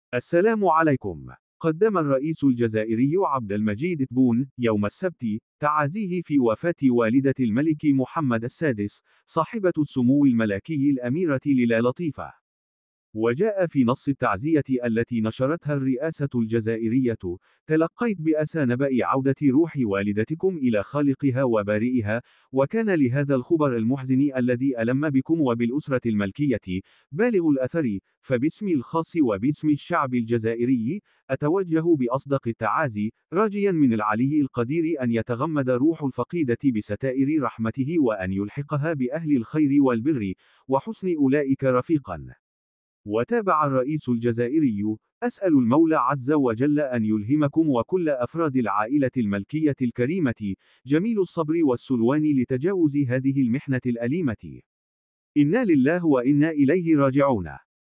تفاصيل رسالة من الرئيس الجزائري للملك محمد السادس
الرئيس الجزائري يعزي الملك محمد السادس في وفاة والدته